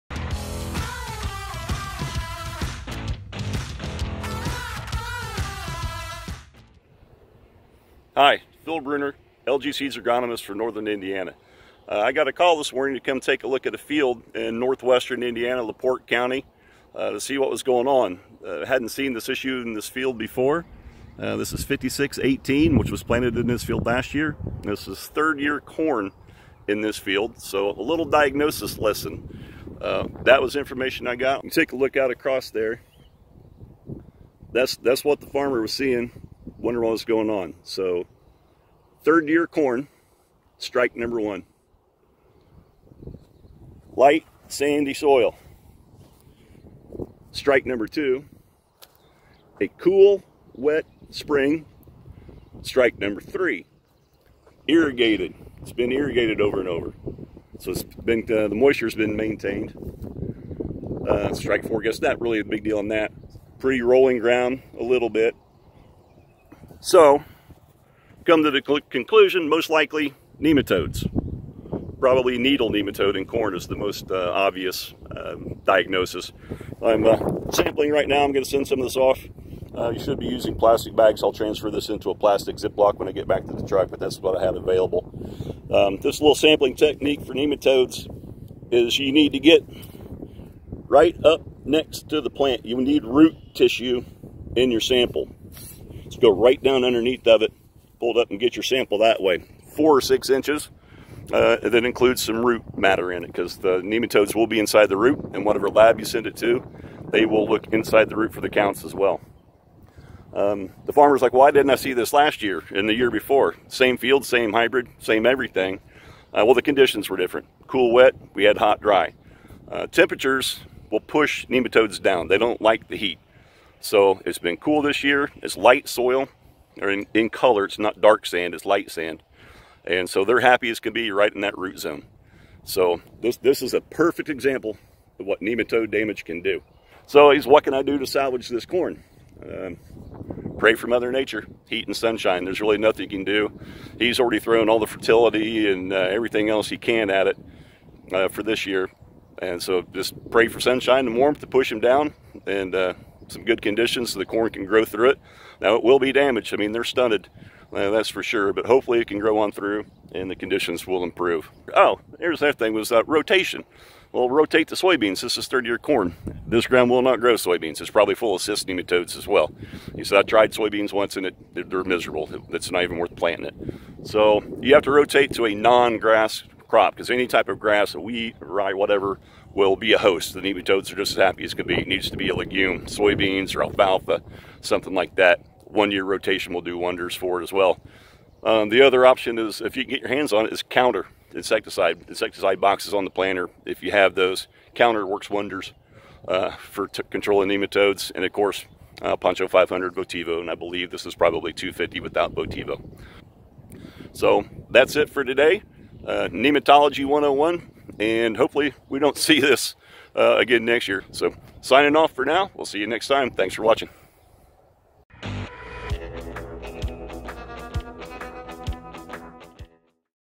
ATOTW: Scouting for Nematodes
Technical Team Agronomist Phil Brunner is out scouting a field - he suspects nematode presence. What're the next steps? Watch here!